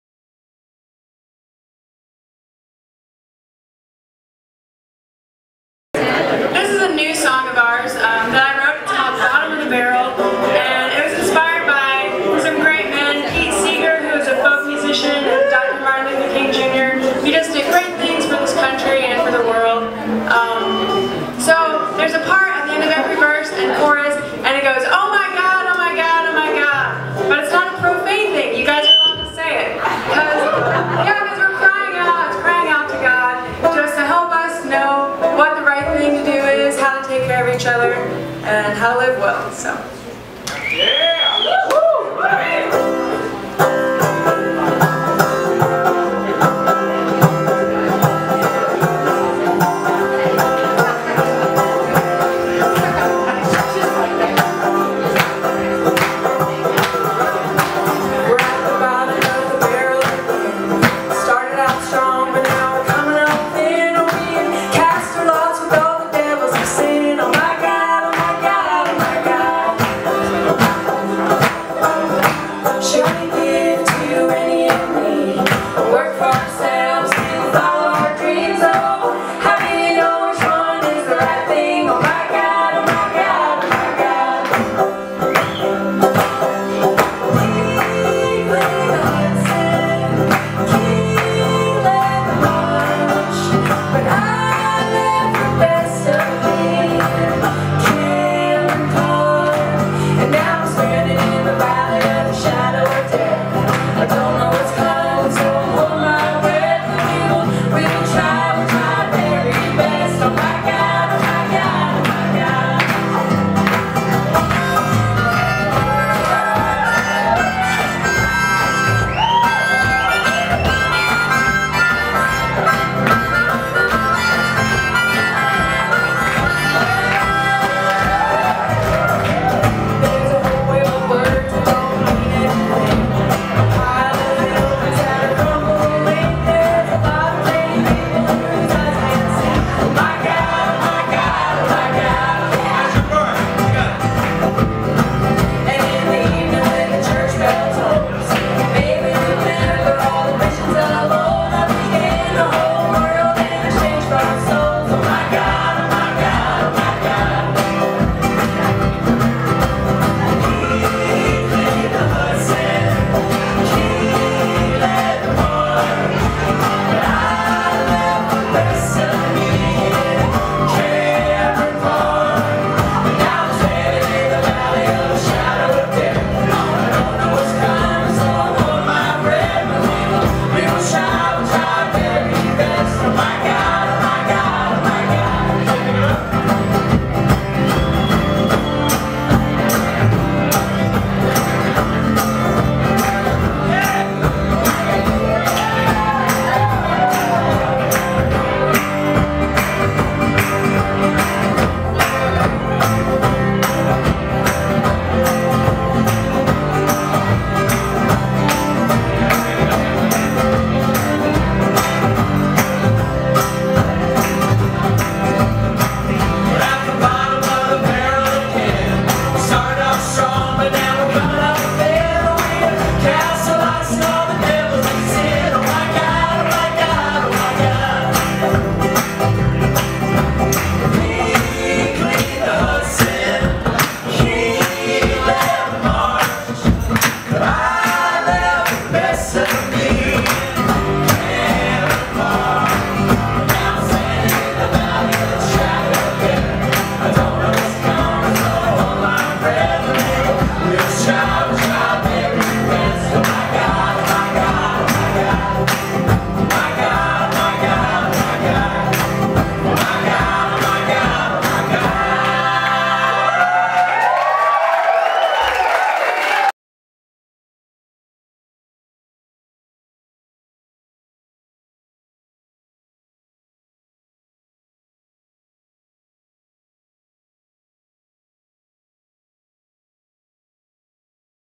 Forlorn Strangers
Happened upon this band when visiting "Clay Glass Metal Stone Gallery" at 15 South J Street, Lake Worth FL 33460; they were playing across the street at "Common Grounds Coffee Bar". The Band is called "Forlorn Strangers” and they are awesome